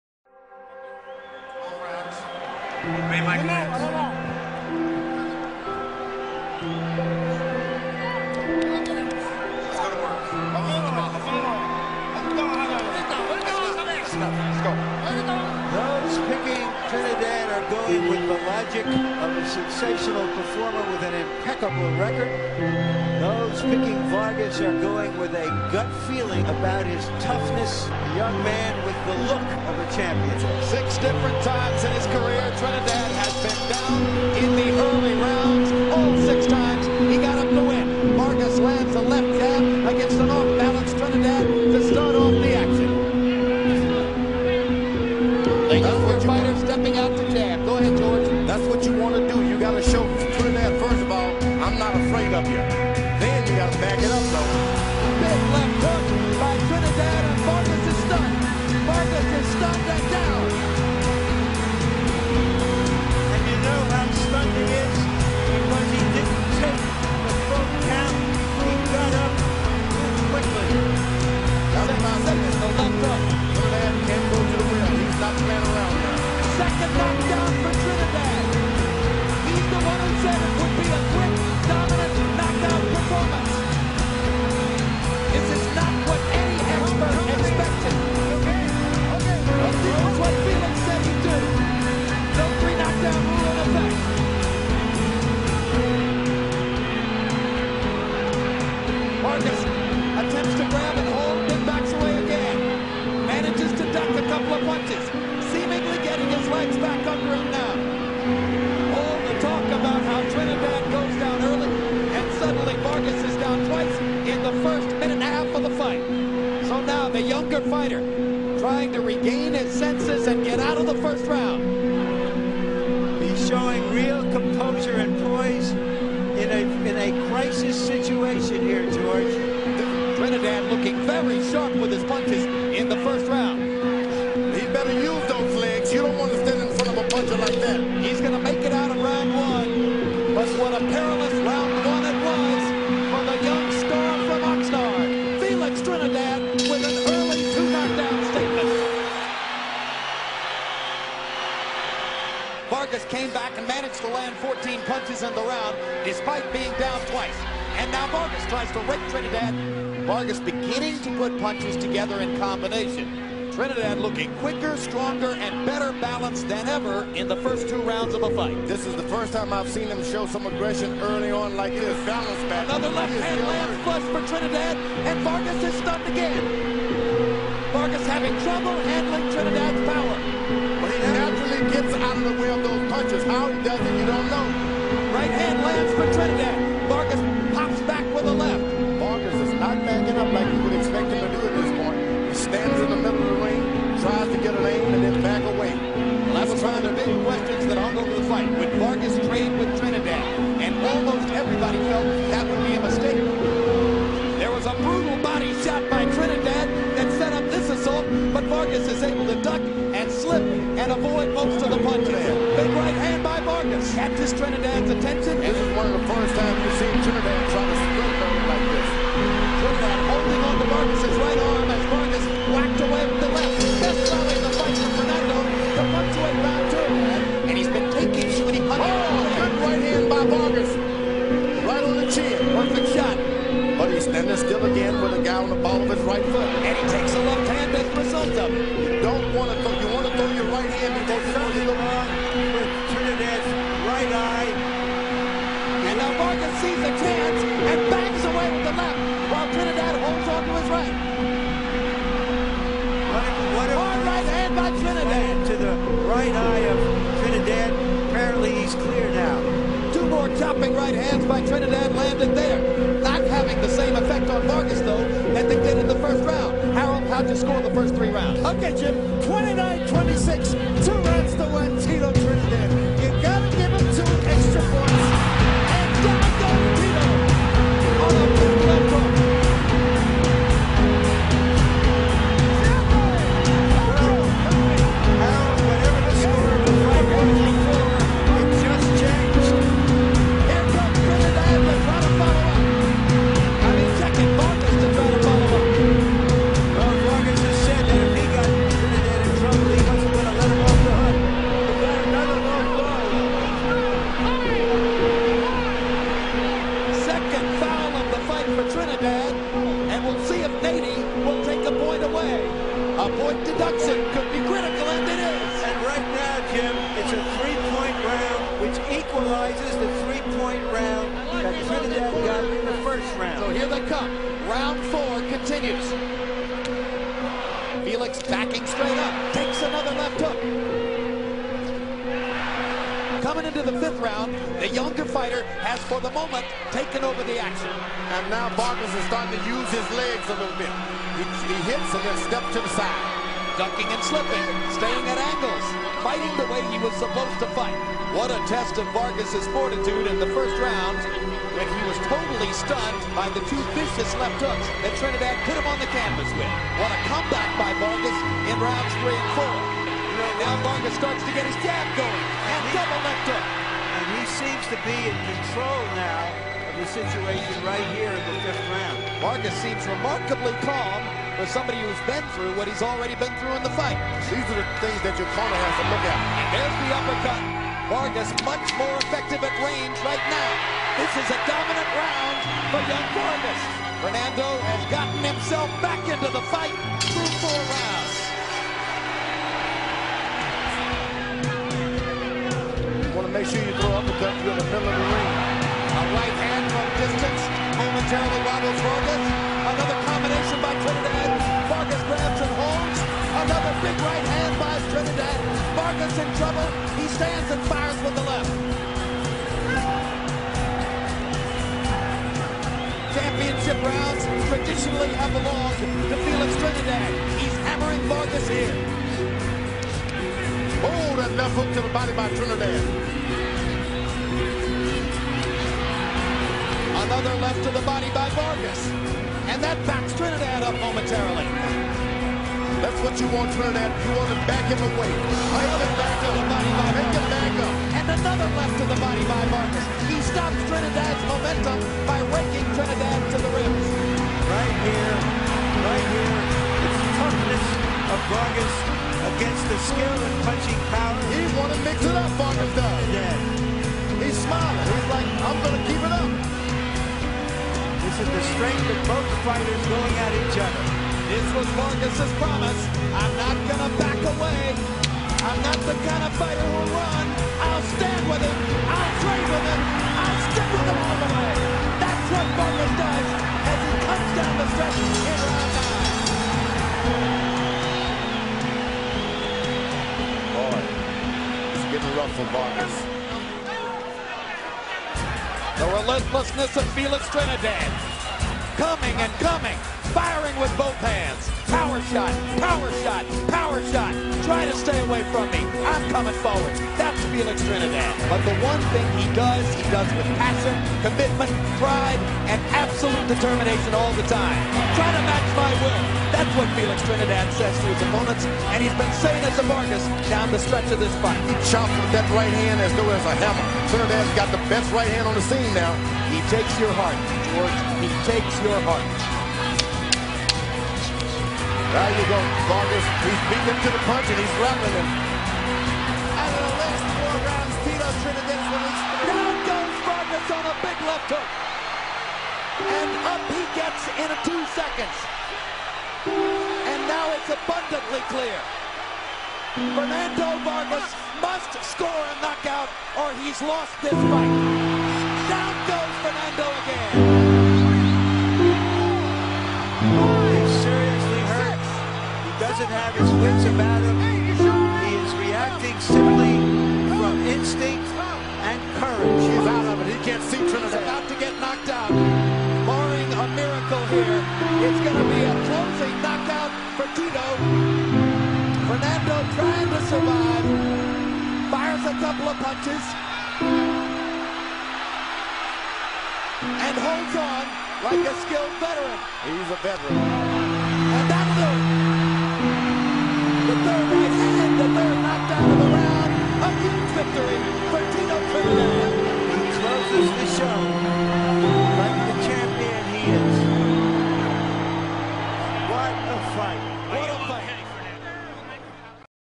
Felix Trinidad vs Fernando Vargas
watch the epic highlights of this boxing war between Trinindad vs Vargas- highlights provided by Gorilla Production GP